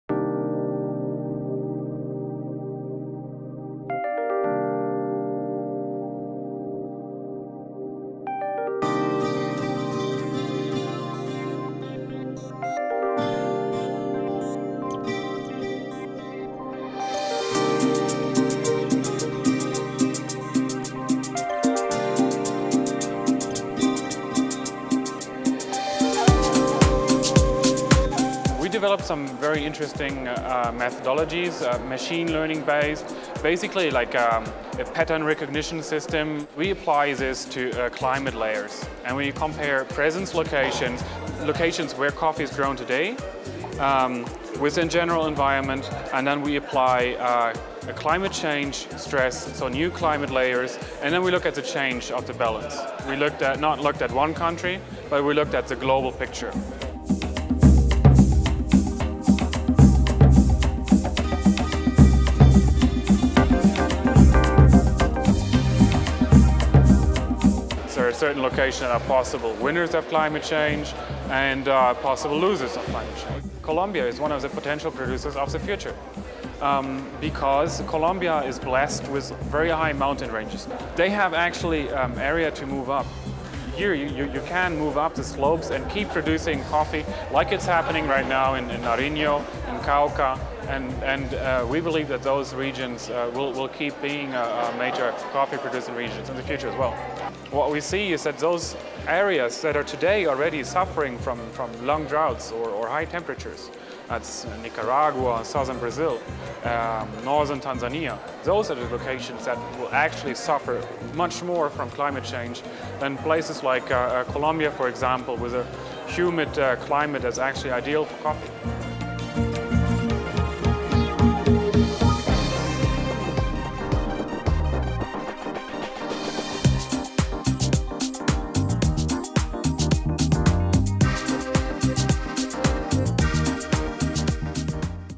How Climate change will affect Coffee Production
An special guest of ASIC 2014 was Dr Christian Bunn who worked with the International Center for Tropical Agriculture (CIAT) and performed a multi-level simulation to predict at a global scale where coffee would be suitable for plantation some years from now taking in account the Climate Change.

It turns out that Brazil, may have a very limited area suitable for production, while East Africa or Colombia are places that will more easily be able to mitigate the effects of climate changes.

It is indeed a topic that every coffee professional out there will want to know about. Watch the video and share us your comments.